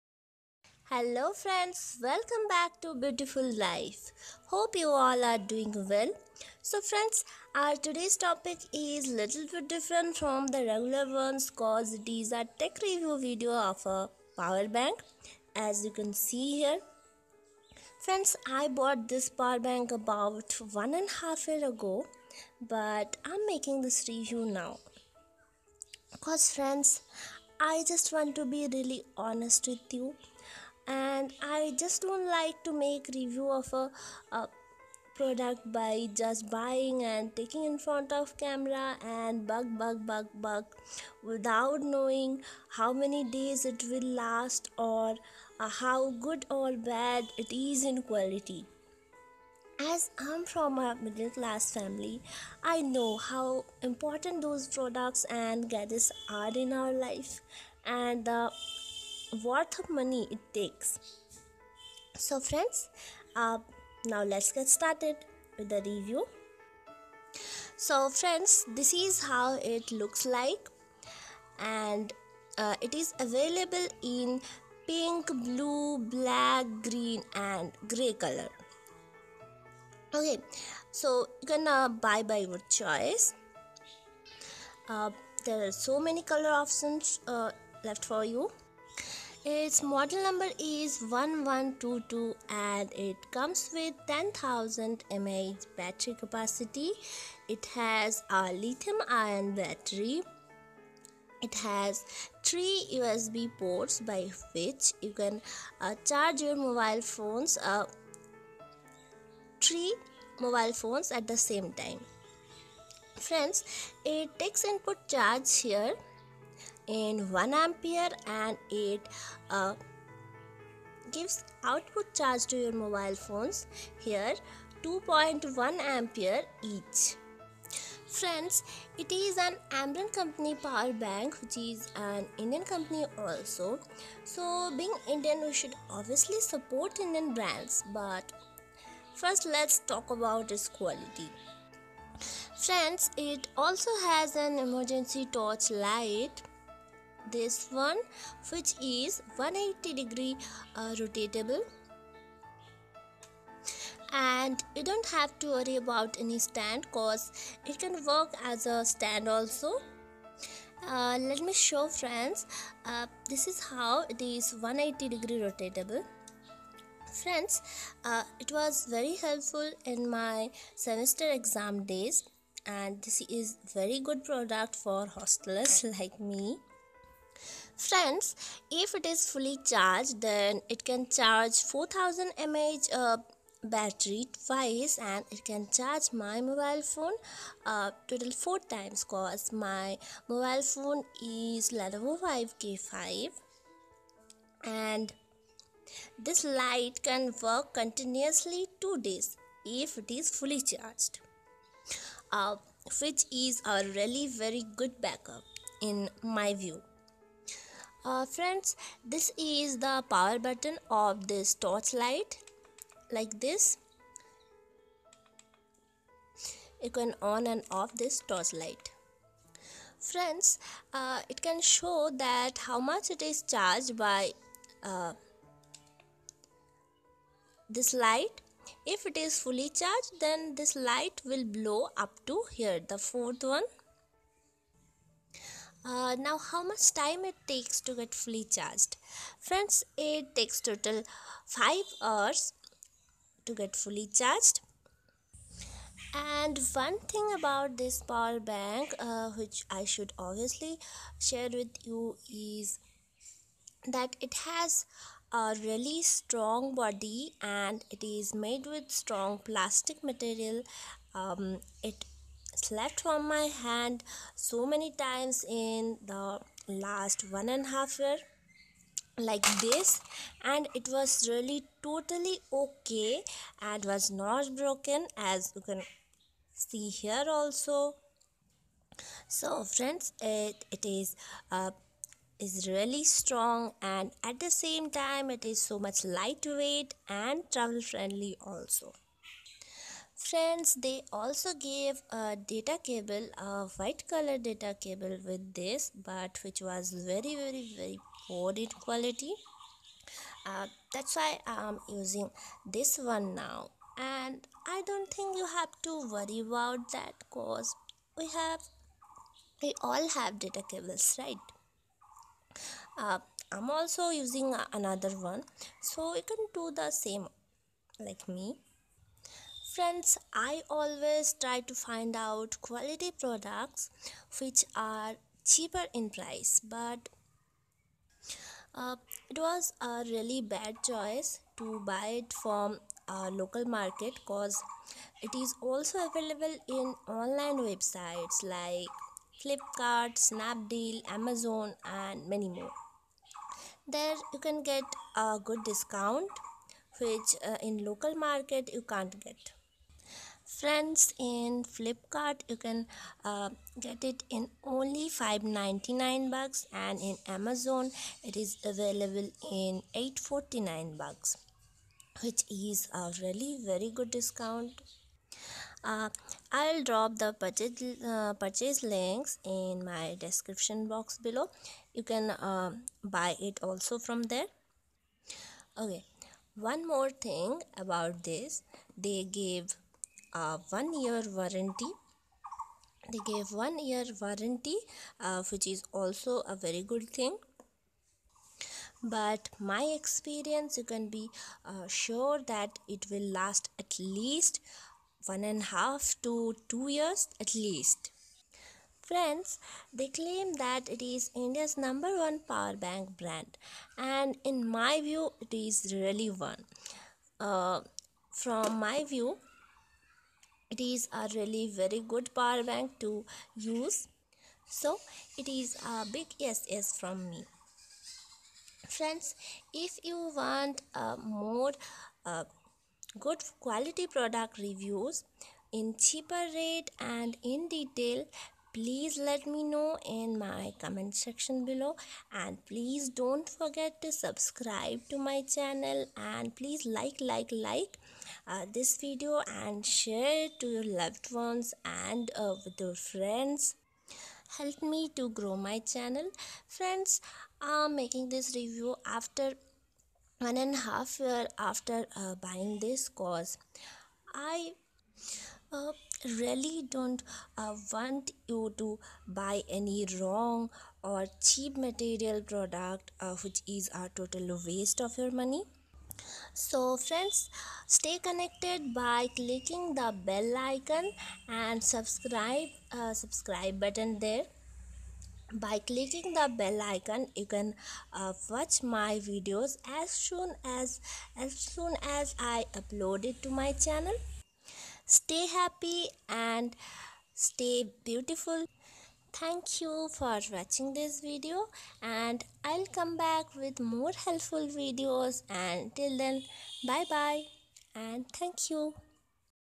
Ambrane p-1122 10000 mah power bank review || Ambrane power bank under 1000 unboxing
Purchase link

¶Ambrane 10000 mAh Power Bank (P-1122, NA)

¶Ambrane P-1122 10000mAH Power Bank (Blue-White)

✓VLCC Cold Cream the Best Cold Cream in the market